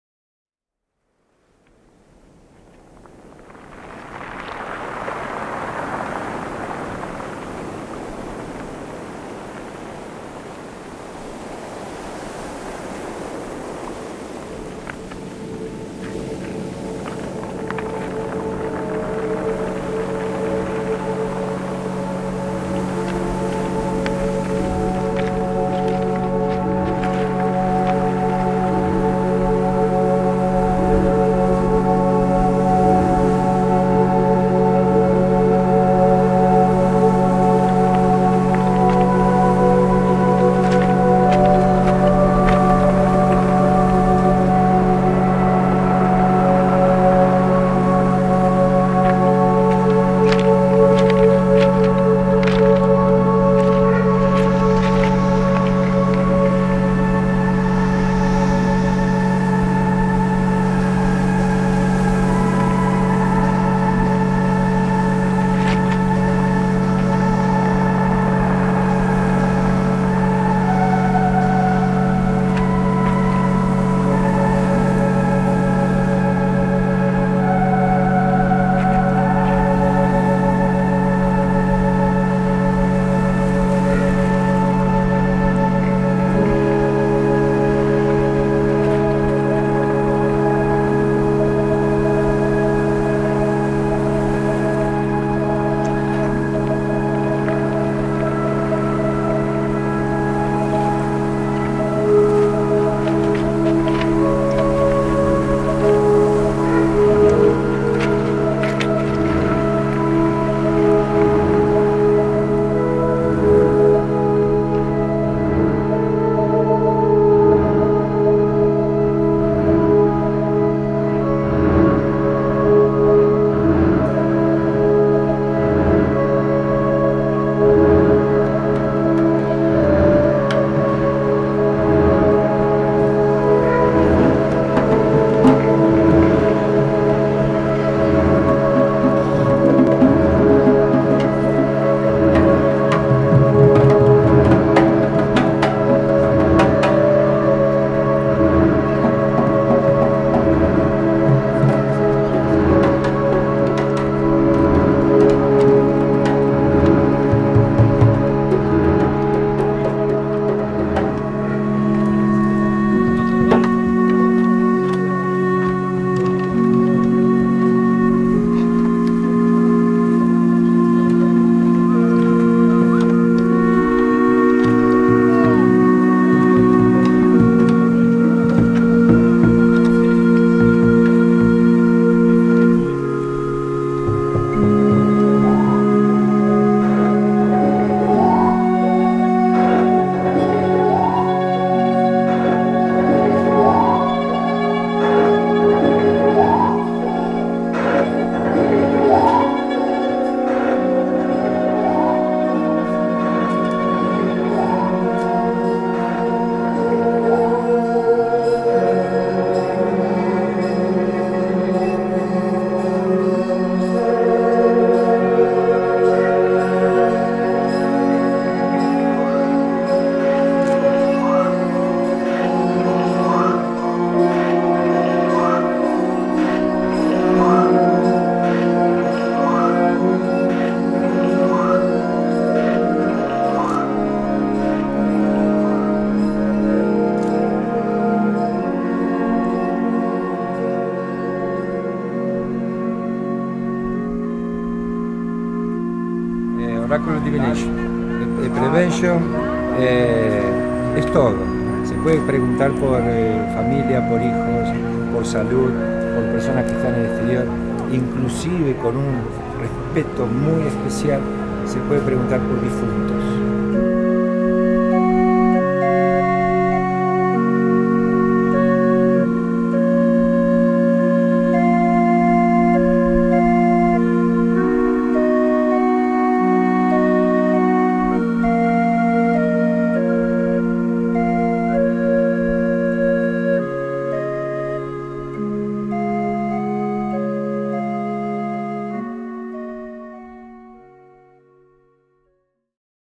Saudaá Group - Uruguay
Saudaá Group questions the poetic dimension of music through accomodating acoustic sets, through using the automatic organ in a contemporary approach, and through producing powerful, repetitive sound textures.  From the begining to the end of 2016, Saudaá Group was involved in a one-year-long tour with his barrel organ to experiment with locals, update a 3-centuries-old instrument and share a music strongly connected to life and "magic".

Music by Alexis Paul, played along with José Pedro Romero (cello) and Diego Adbul Galceran fernandez (portastudio) on march 2016 in Montevideo, Uruguay, during a residence at Espacio Uuu, Pera De Goma. An extract for Label Pop, France Musique.

Photograhy : Alexis Paul